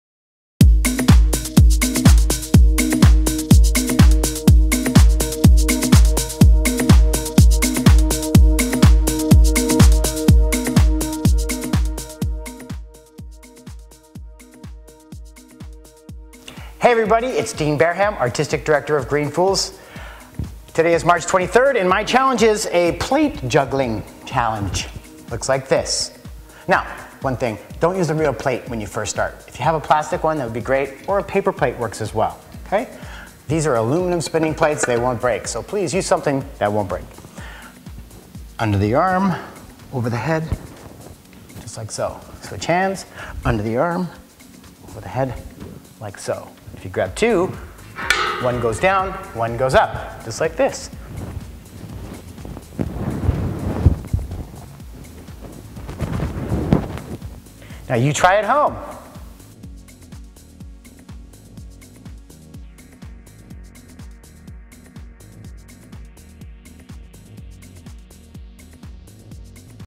Green Fools Daily Challenge March 23rd
You've heard of juggling balls but have you heard of juggling plates?

Give plate juggling a try and let us know how it goes! Make sure to use paper or plastic plates!

Tag us at Green Fools Theatre Society and we'll share your juggling!

Today is our second last workshop!